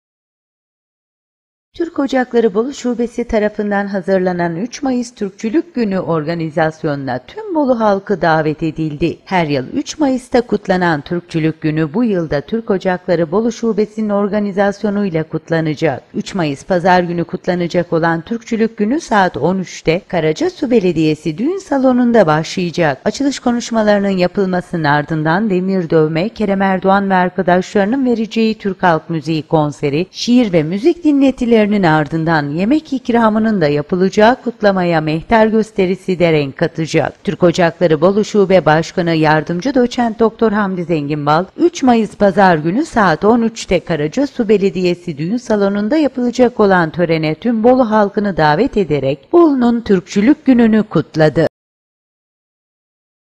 DEMİRLER DÖVÜLECEK, SOFRALAR BEREKETLENECEK…
Türk Ocakları Bolu Şubesi tarafından hazırlanan 3 Mayıs Türkçülük Günün’ne tüm Bolu halkı davet edildi.